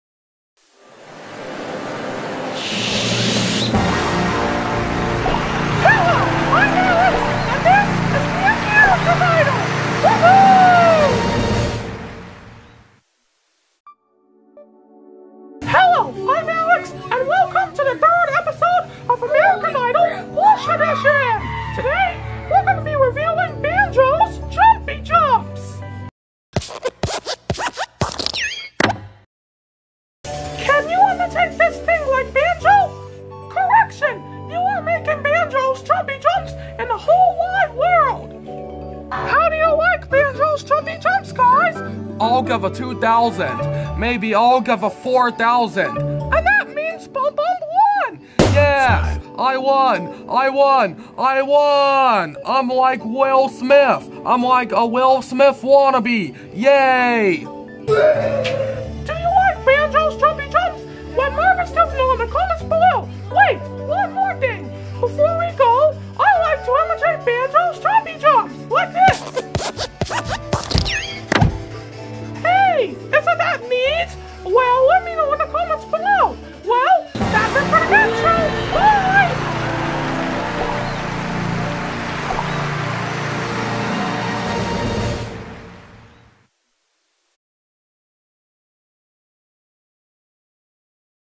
American Idol: Plush Edition - Votes for Banjo's Jumpy-Jumps
Banjo sure loves to jump!

Video made on my Galaxy Tab S7 FE.